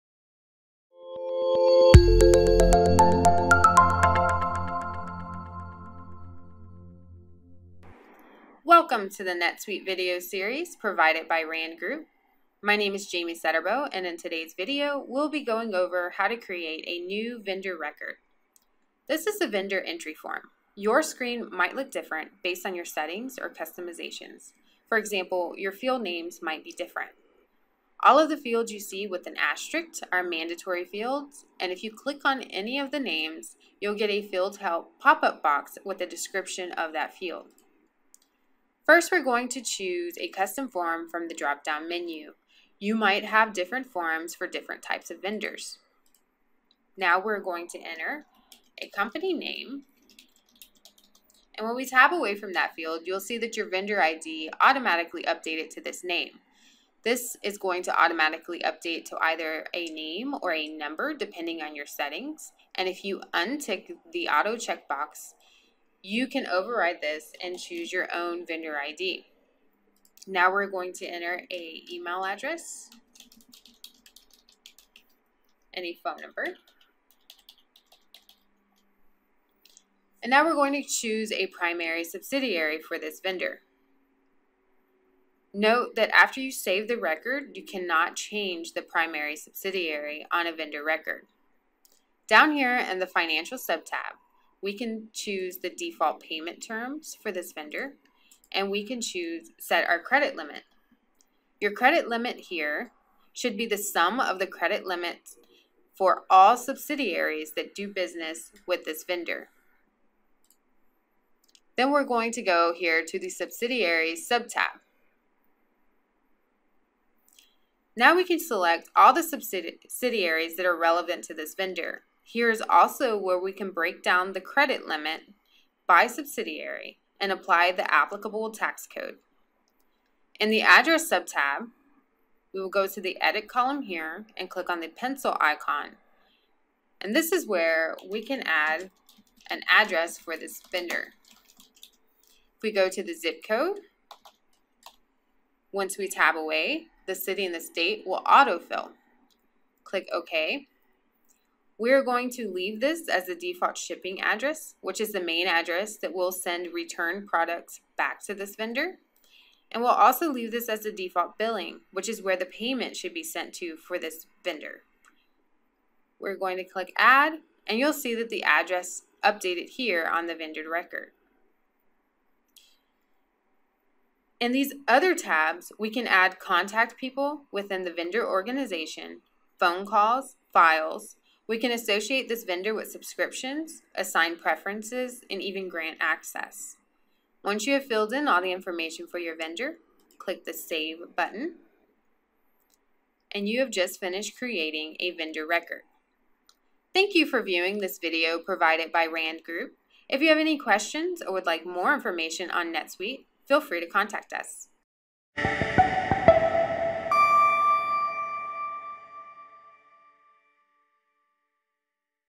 How to Create a Vendor in NetSuite
Learn how to create a vendor in NetSuite ERP.